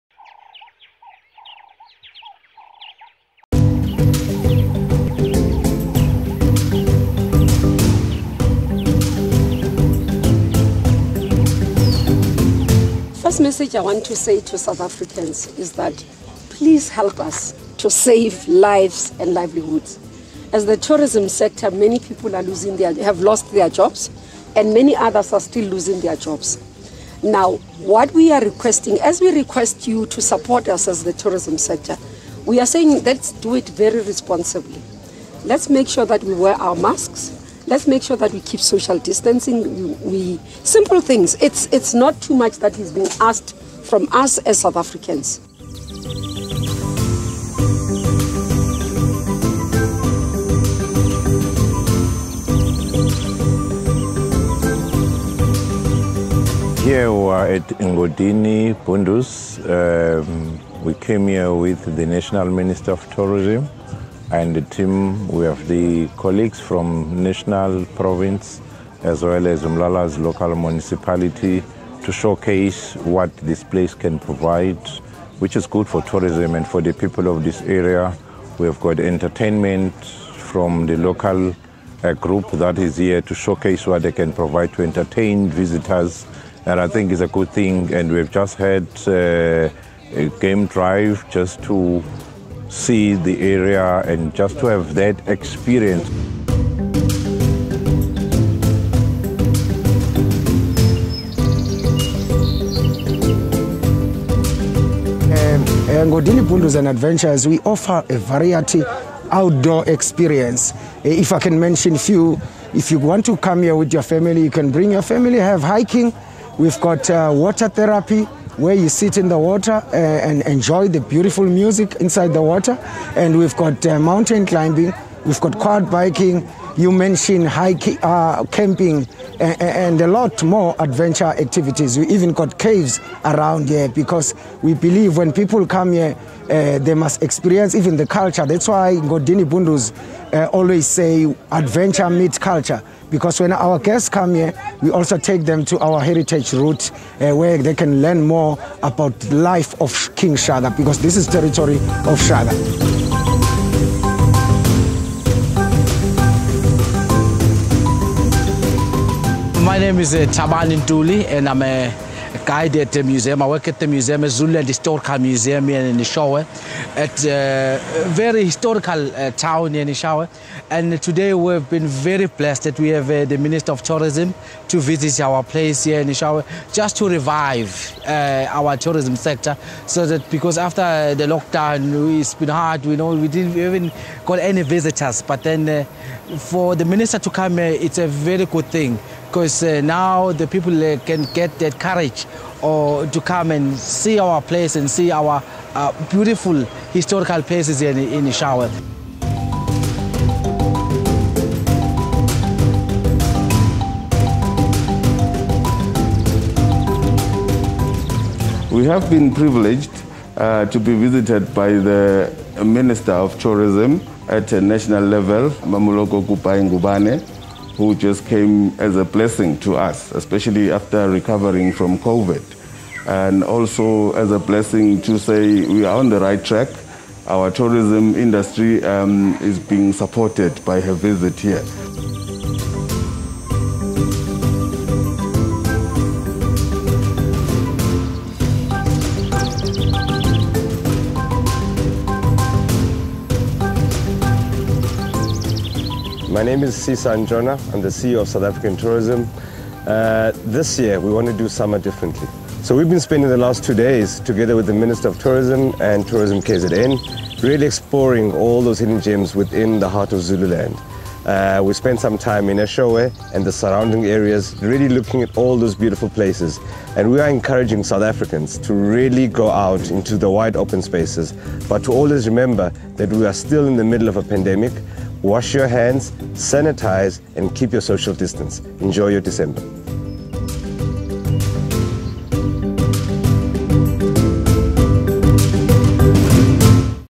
The Minister of Tourism visits King Cetshwayo Municipality on her Domestic Travel Campaign!
Last week we joined the Minister of Tourism- Mmamoloko Kubayi-Ngubane, on part of her domestic travel campaign around South Africa. Along with EDTEA, we visited King Cetshwayo District Municipalty & the Eshowe area in KwaZulu-Natal in order to promote tourism in the area, whilst the Minister promoted clamping down on Covid-19 safety protocols over the festive season.

We were hosted by Aloe Lifestyle Hotel, and visited tourist sites such as Dlinza Forest, Fort Nongqayi Museum, King Cetshwayo’s Memorial site and not to mention a visit to uMlalazi Municipality where we were taken on a guided tour through the Siyaya Coastal Park which is now classified as a protected site. The Eshowe area is no doubt steeped in rich history, being an area which hosted some of the greatest Zulu Kings!

We also visited Ngodini Bunduz, an adventure paradise, where activities such as camping, 4x4 trails, paintball and quad biking is all available.

It was great to see how establishments and tourist attractions are abiding by the rules and regulations set out in order to keep everyone safe, and this campaign was evidence that tourism can still be promoted whilst adhering to safety rules.